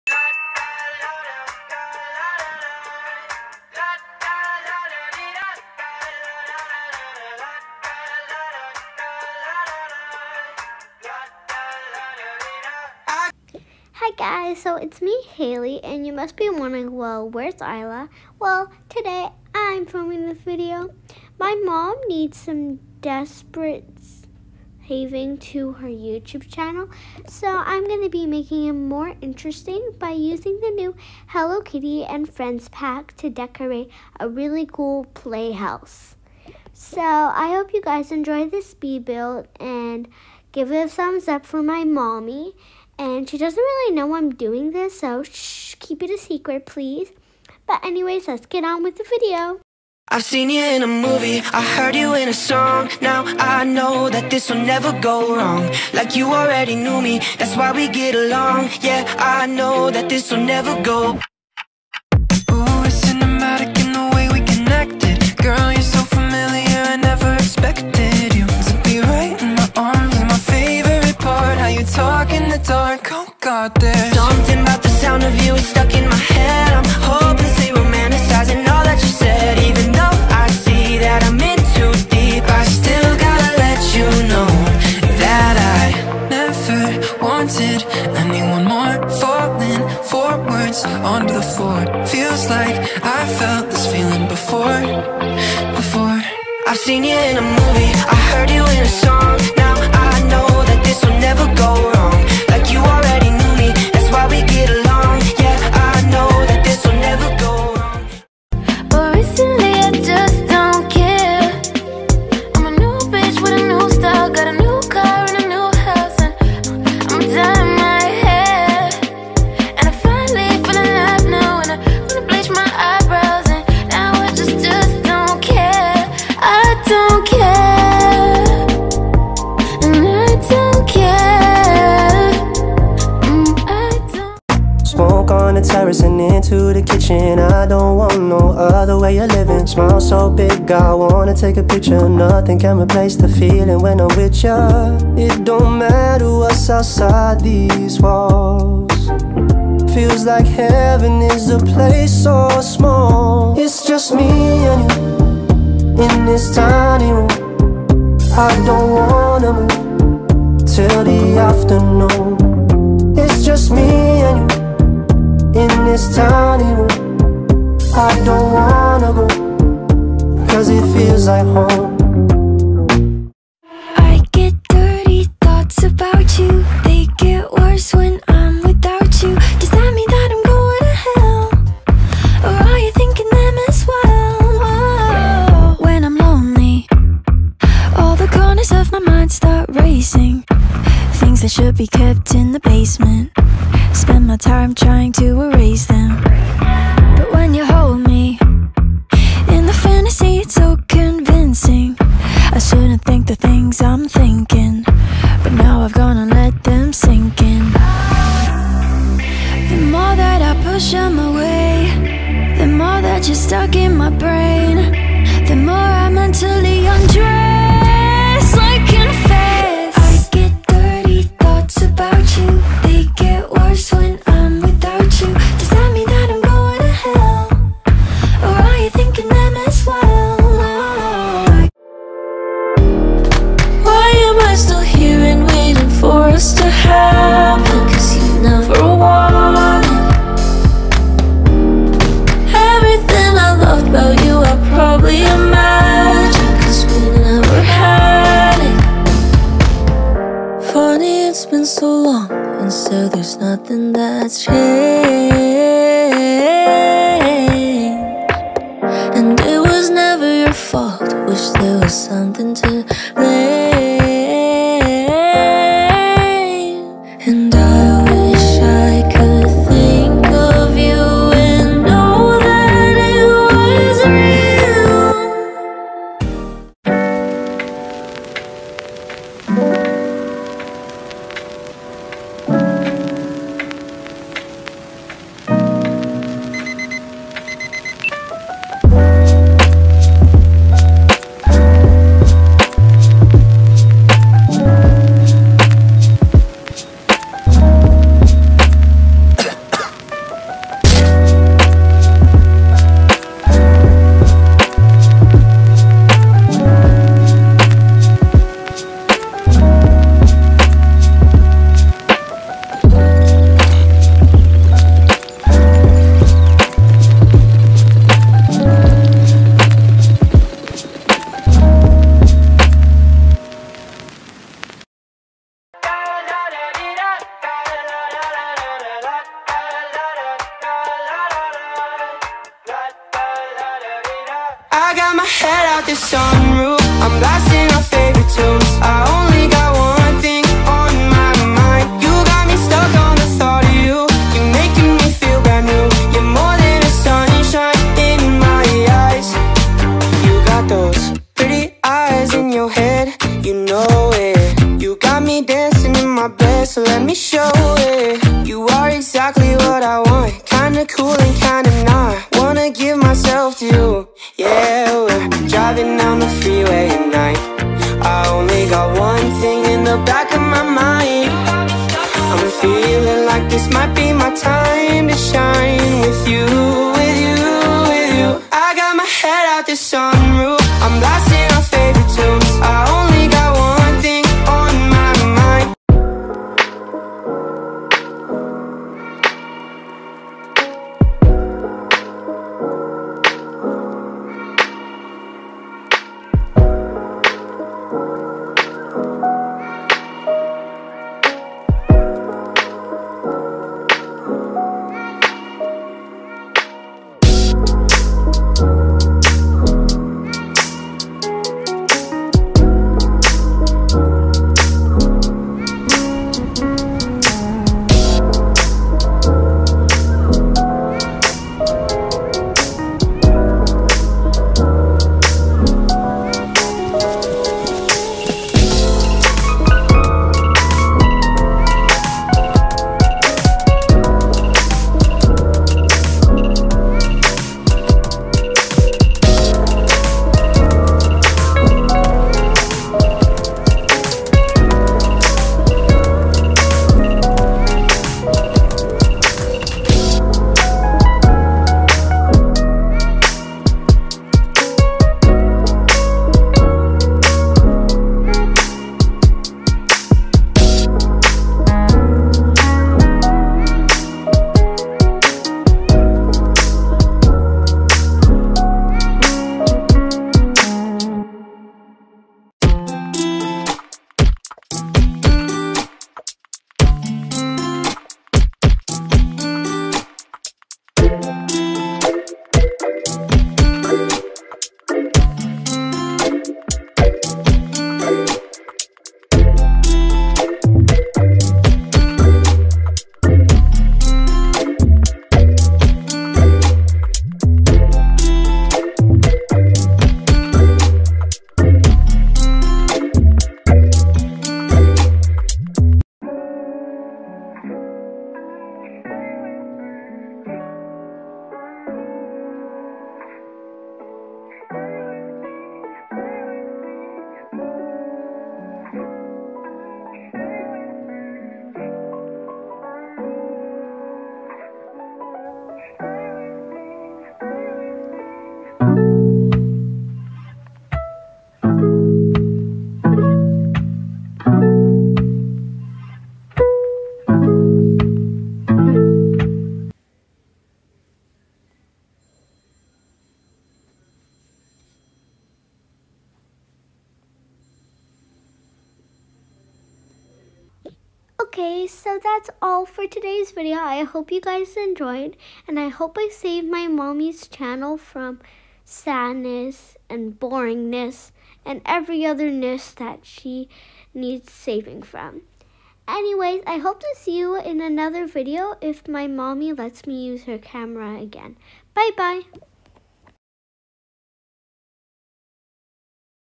/Decorating a Hello kitty and friends play house🏡/*SPEED-BUILD*💨/
(⁄ ⁄•⁄ω⁄•⁄ ⁄)⁄☾˙❀‿❀˙☽
🌈 ♡༉ ❱ ꒰⸝⸝•ᴗ•⸝⸝꒱ 𝓟ᜊ𝐒𝐓 💟๑˚· ꒱ ୧ ⋆˚
:¨·.·¨: 𝙳𝙰𝚃𝙴 : 17 / 07 / 2022
`·. 𝙶𝙾𝙰𝙻 : 1,000! :))
〔 🍰⁺ ✶˚🍋 *๓´💐
Hiya, today I’ll be using the hello kitty and friends pack to decorate the bohemian house. The end results came out pretty good. Free to use!
╰─➤𝙲𝚘𝚖𝚖𝚎𝚗𝚝 𝚟𝚒𝚍𝚎𝚘 𝚒𝚍𝚎𝚊𝚜
𝚃𝚑𝚡 𝚜𝚘 𝚖𝚞𝚌𝚑 𝚏𝚘𝚛 𝚠𝚊𝚝𝚌𝚑𝚒𝚗𝚐 ☆
-👀𝚁𝚎𝚖𝚎𝚋𝚎𝚛-
𝚆𝚑𝚊𝚝 𝚎𝚍𝚒𝚝𝚗𝚐 𝚊𝚙𝚙𝚜 𝙸 𝚞𝚜𝚎
-𝚒𝚗𝚜𝚑𝚘𝚝
-𝚌𝚊𝚗𝚟𝚊
-𝖢𝖺𝗉𝖢𝗎𝗍